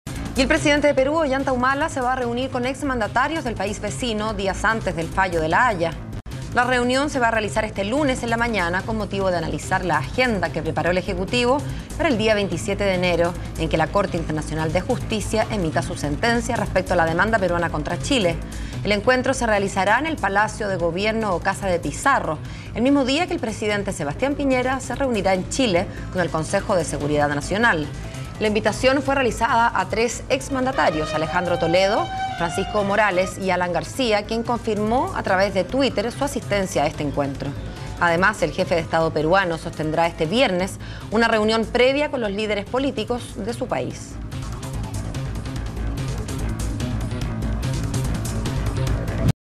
Ollanta Humala se reunirá con Expresidentes del Perú, días antes del fallo de La Haya
Además, el jefe de Estado peruano, sostendrá este viernes una reunión previa con los líderes políticos de su país.

El Presidente Ollanta Humala sostendrá un encuentro con los exmandatarios del país vecino el próximo lunes en la mañana, con motivo de analizar la agenda que preparó el Ejecutivo para el día 27 de enero en que la Corte Internacional de Justicia, emitirá su sentencia respecto a la demanda peruana contra Chile.

El encuentro se realizará en el Palacio de Gobierno o Casa de Pizarro, el mismo día que el Presidente Sebastián Piñera se reunirá en Chile con el Consejo de Seguridad Nacional.
La invitación fue realizada a tres Expresidentes, Alejandro Toledo, Francisco Morales y Alan García, quien confirmó a través de Twitter su asistencia al encuentro.